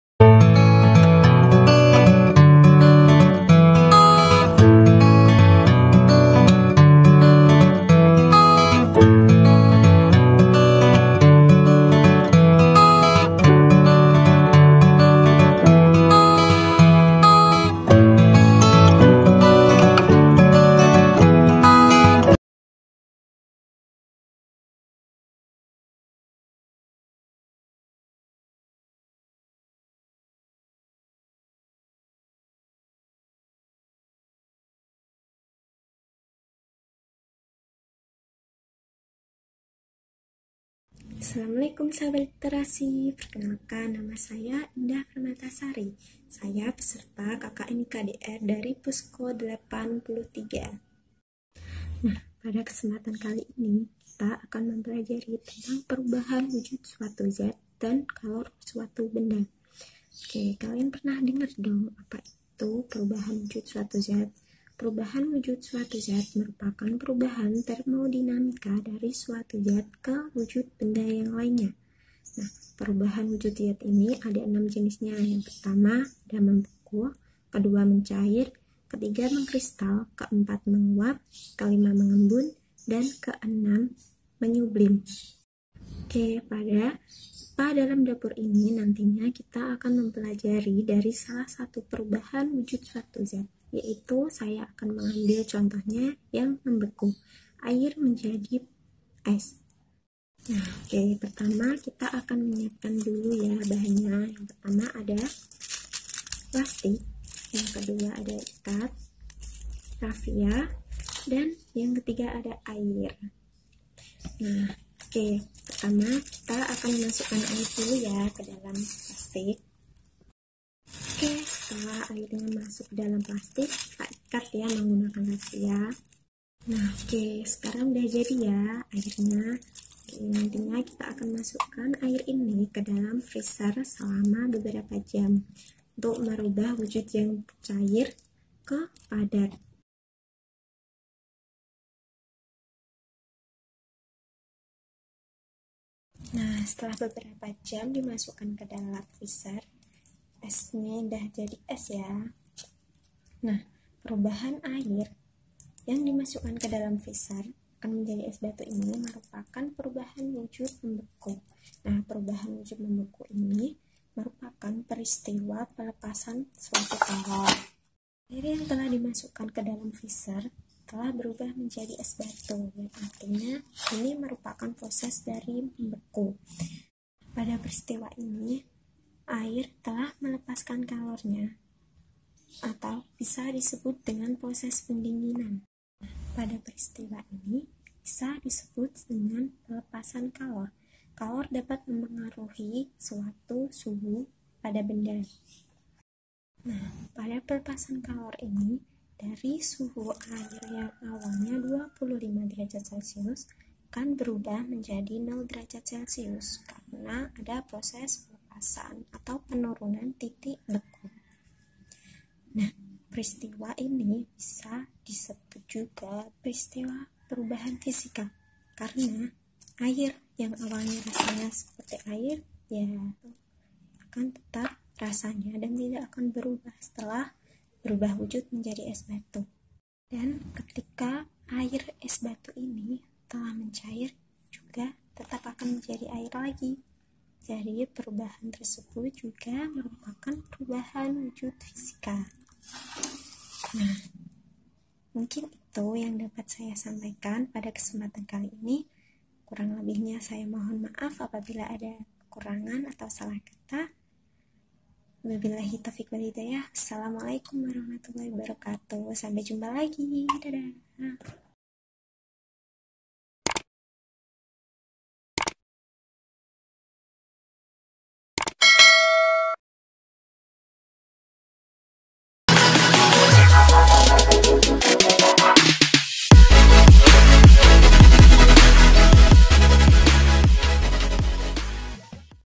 Ada Sains Dalam Es Batu | KKN-IK-DR IAIN KUDUS | Tadris IPA | Kelompok 83
Dipublikasikan tanggal 06 juli 2020

Nama vlogger: Indah Permata Sari
DPL: Dody Rahayu Prasetyo, M.Pd

Vlog ini dapat digunakan sebagai sumber belajar Kelas 7
Kompetensi Dasar
3.3 Menjelaskan konsep campuran dan zat tunggal (unsur dan senyawa), sifat fisika dan kimia, perubahan fisika dan kimia dalam kehidupan sehari-hari.
3.4 Menganalisis konsep suhu, pemuaian, kalor, perpindahan kalor, dan penerapannya dalam kehidupan sehari-hari termasuk mekanisme menjaga kestabilan suhu tubuh pada manusia dan hewan.